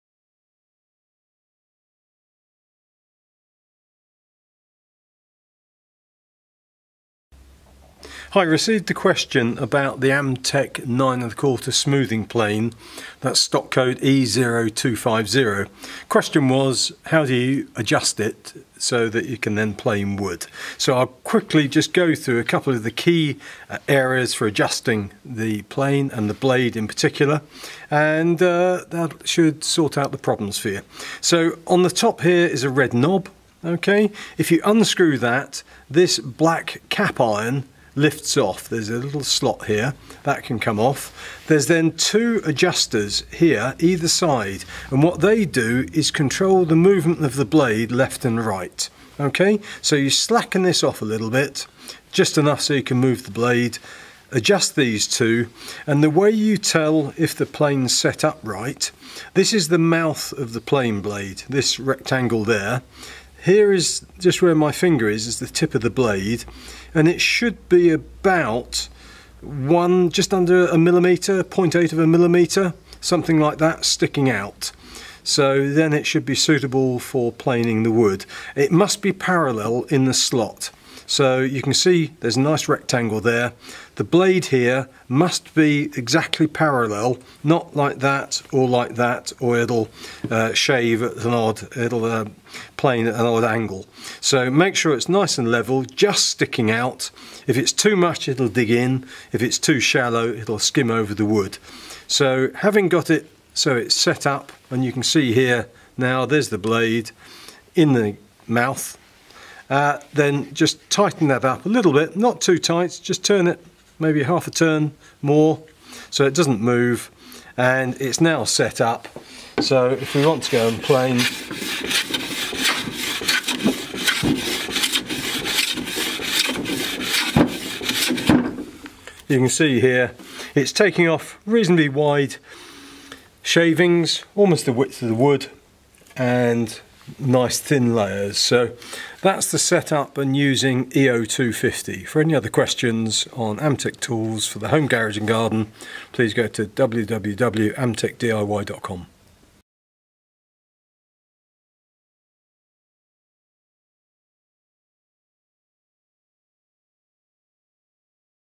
How do you adjust a smoothing plane?
In this video our DIY expert will answer questions sent by you directly from our website. Today he is using a smoothing plane (Stock Code: E0250).